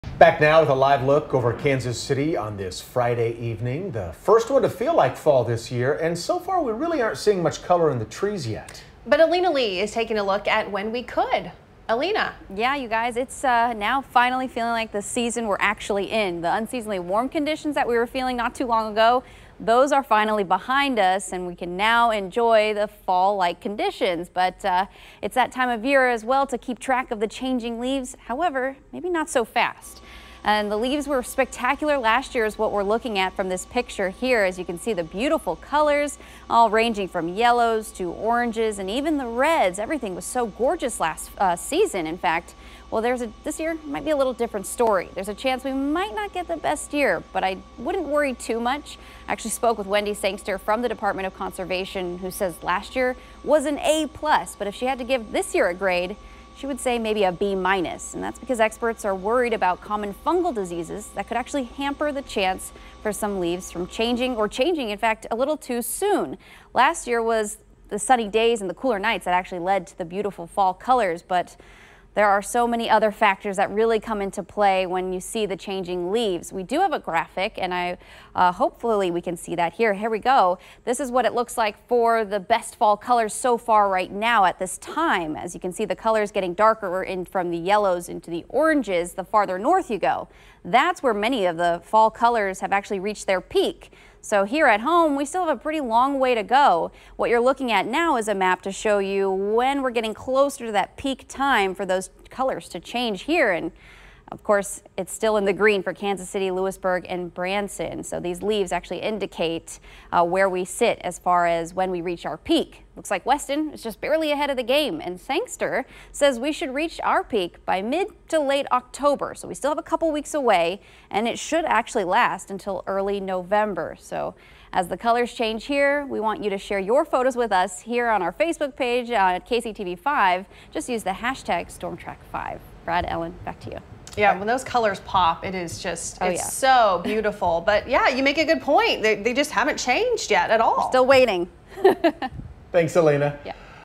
KC metro may not get best fall foliage this year
Now that the unseasonably warm temperatures are behind us and it's finally feeling like fall, it's that time of year to keep track of the changing leaves. But, not so fast.